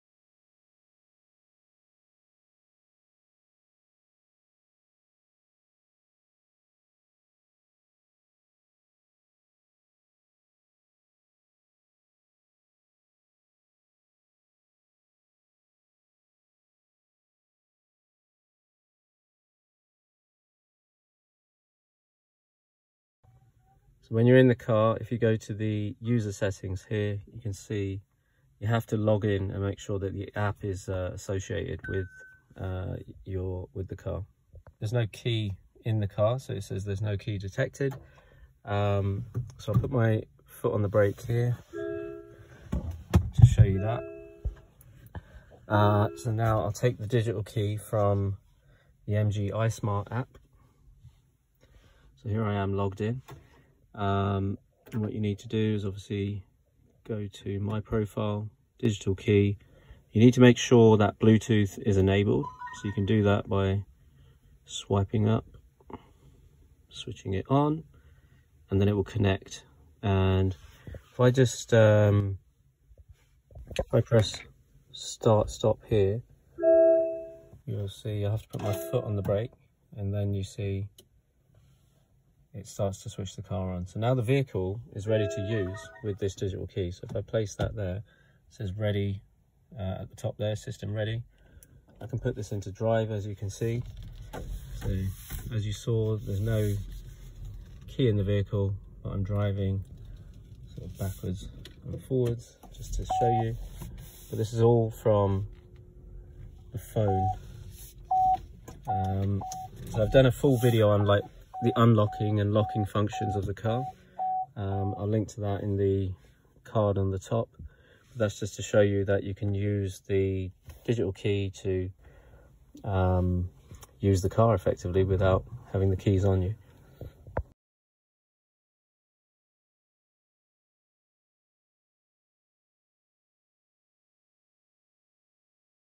2023 MG4 Digital Key
A quick how to video on how to set up the bluetooth digital key on the MG4 using the MG iSmart app.  Some people have had this feature removed from their apps but it is available on the 2023 model.  This was recorded in October 2023. The Digital key is free to use and can be shared with other family members or friends.